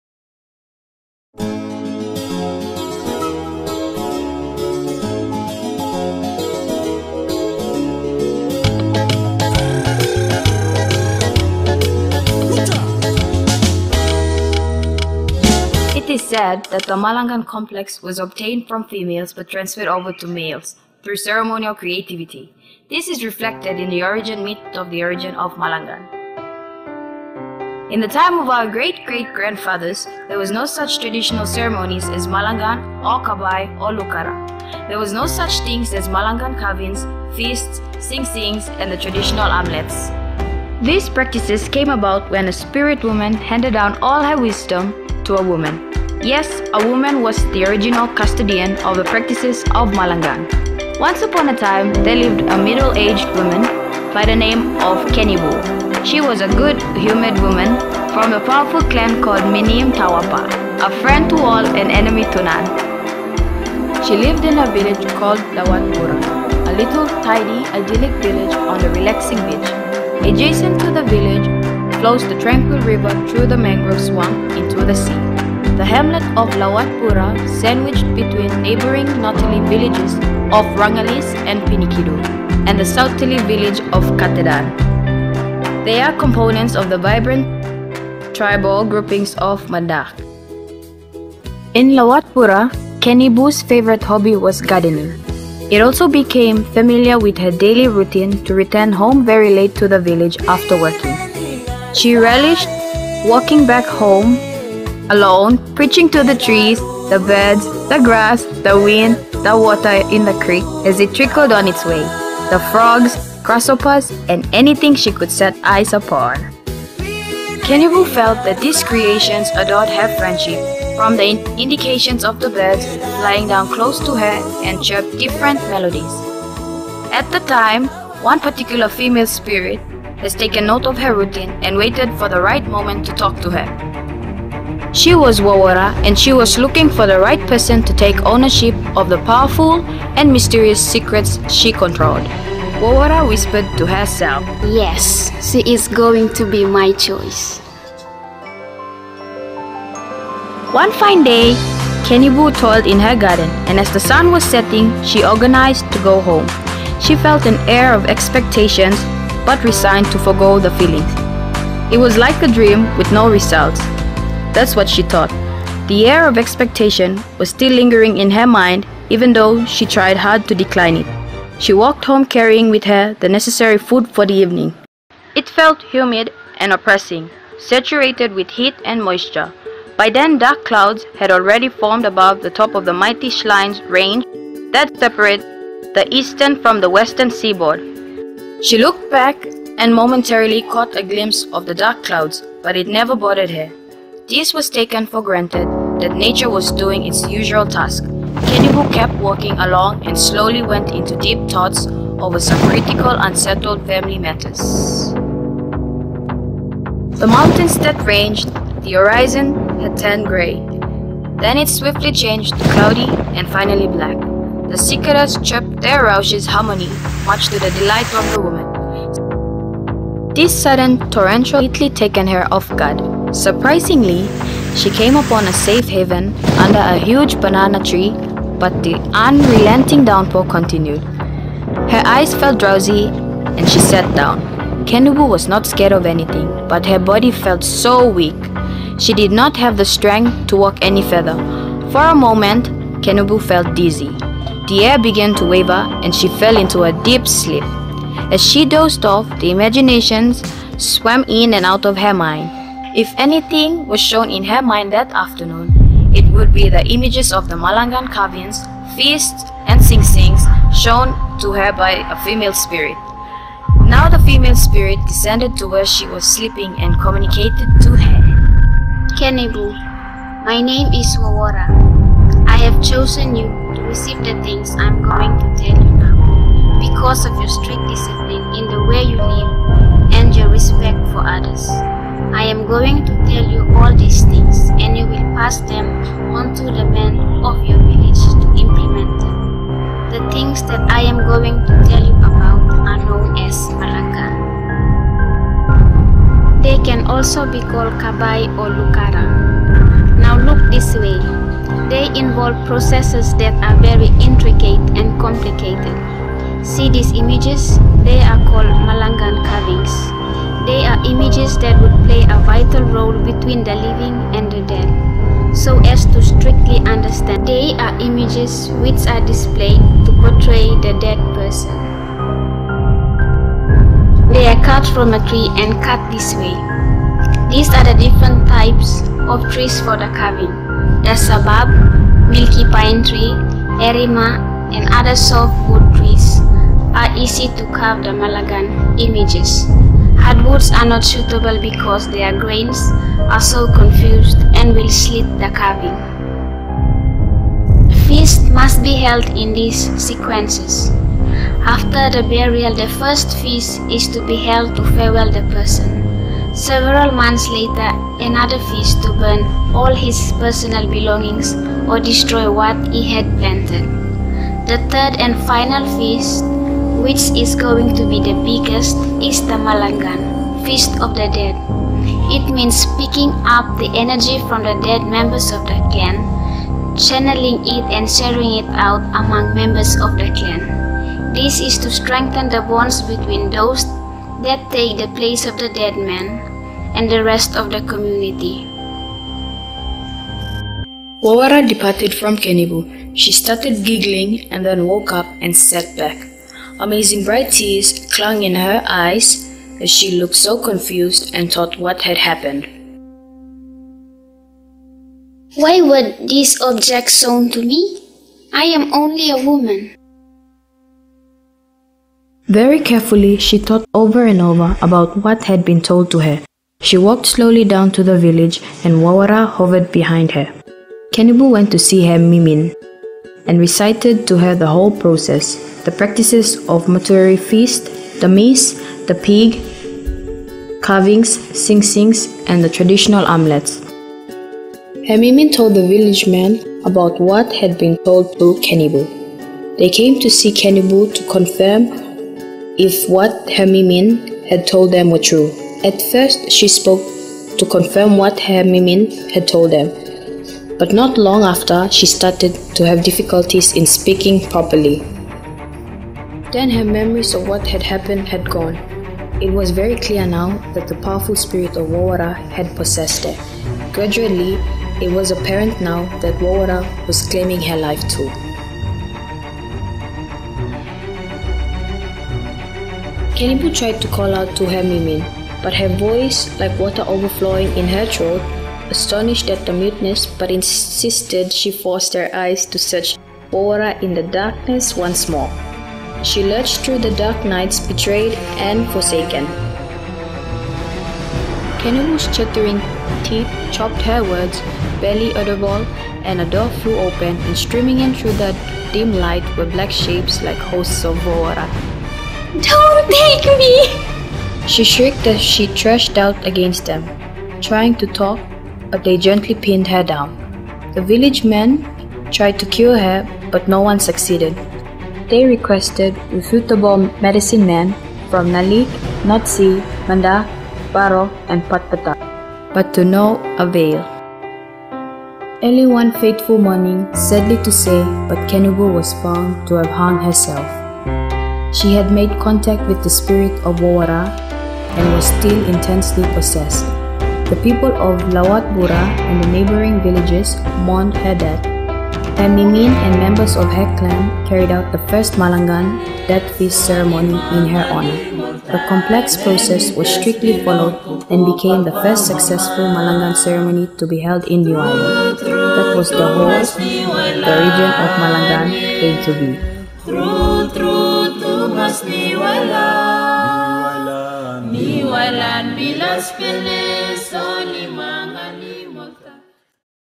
Origin of Malangan - DWU New Ireland Students Association
Drama acted by the New Ireland students of the Divine Word University about the Origin of the Malanngan in the New Ireland Province.
written by Livingko